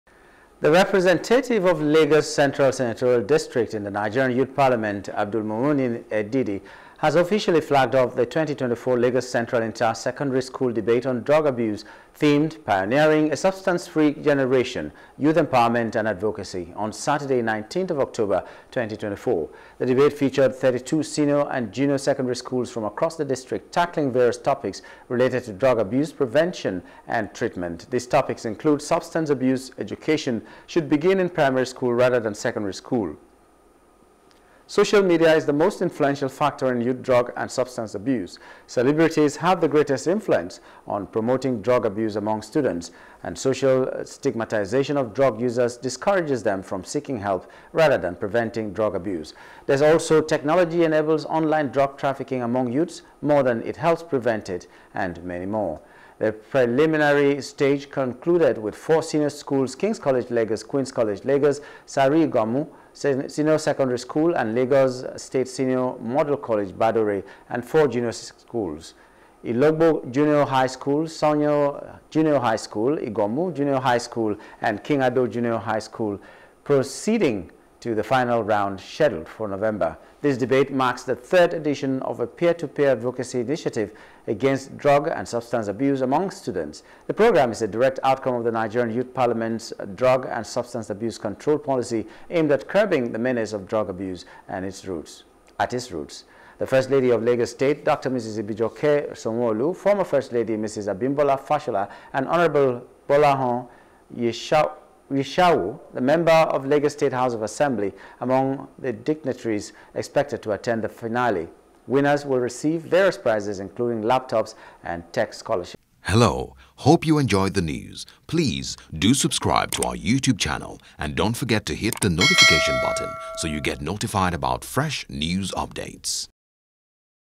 Education Lagos Central Senatorial District Flags Off 2024 InterSecondary School Debate On Drug Abus
Education Lagos Central Senatorial District Flags Off 2024 Inter-Secondary School Debate On Drug Abuse